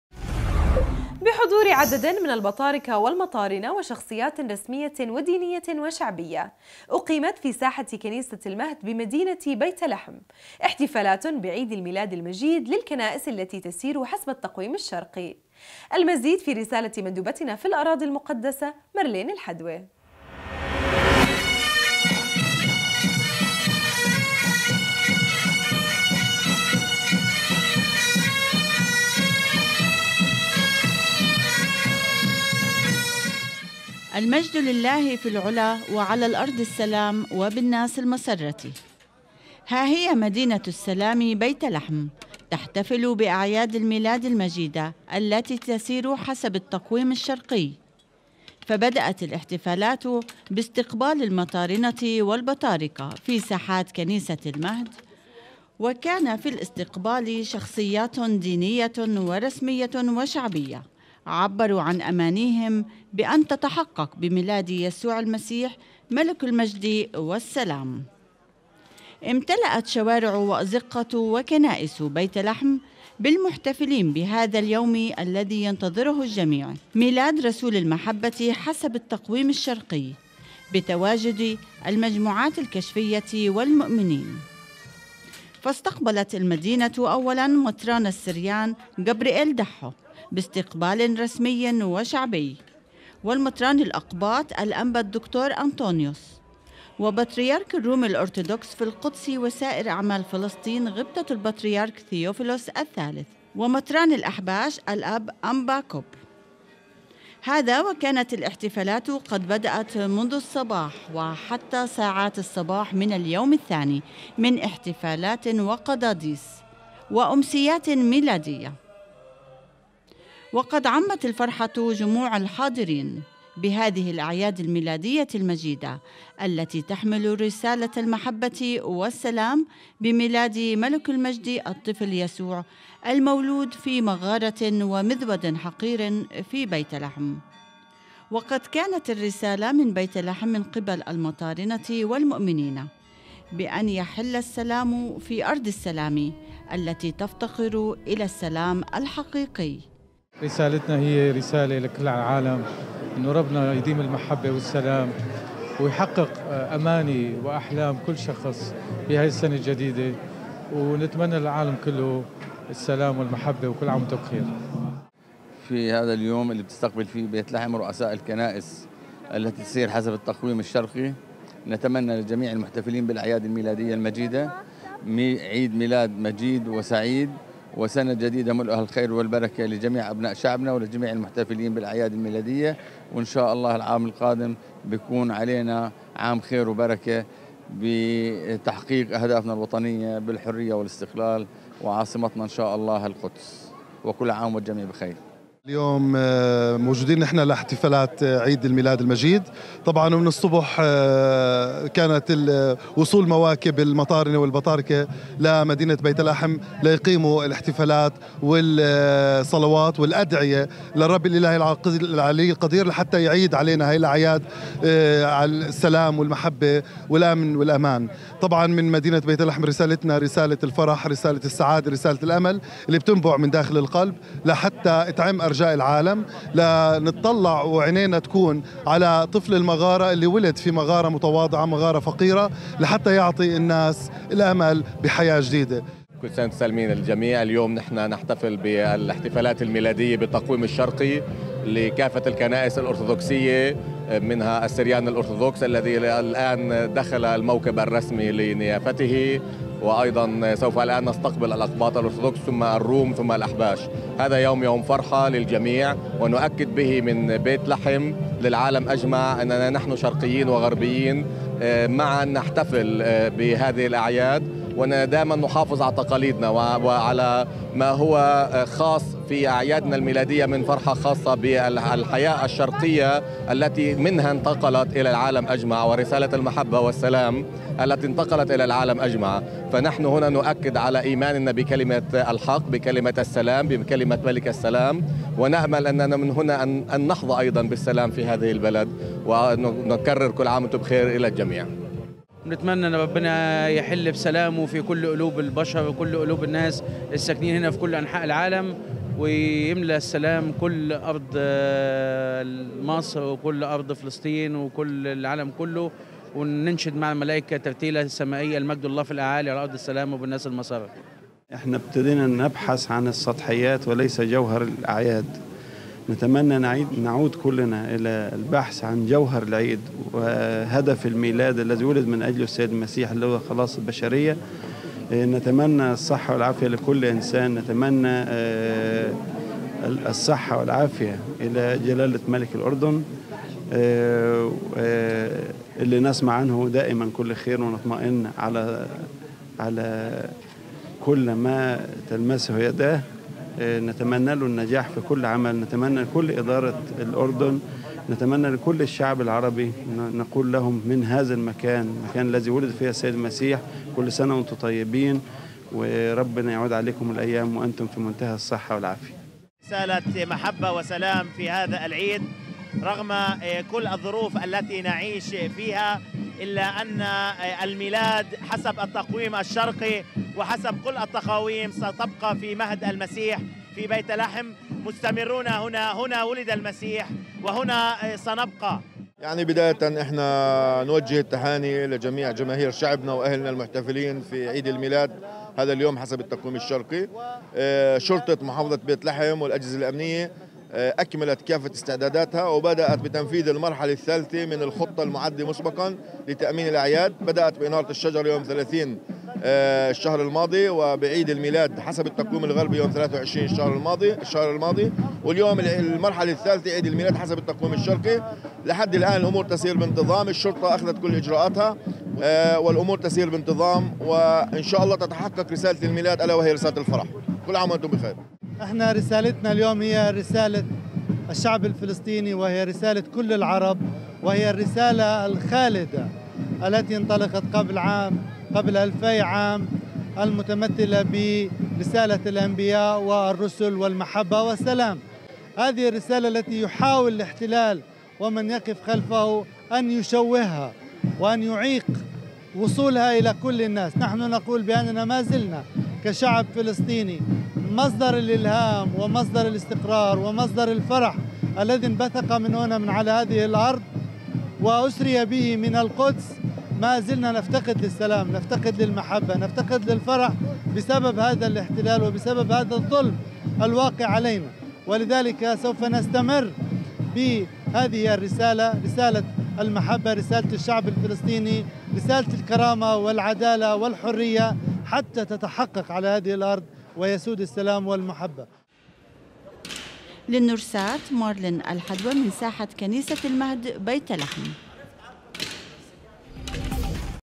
نورسات الأردن : احتفالاتُ العيدِ المجيد حسب التقويم الشرقي تعمُّ بيتَ لحم
بحضورعددٍ من البطاركةِ والمطارنة وشخصياتٍ رسمية ٍودينيةٍ وشعبية / ، أقيمت في ساحةِ كنيسةِ المهد بمدينةِ بيتَ لحم / احتفالاتٌ بعيدِ الميلادِ المجيد للكنائسِ التي تسيرُ حسبَ التقويمِ الشرقي / .. المزيد في رسالة مندوبتنا في الاراضي المقدسة مارلين الحدوه .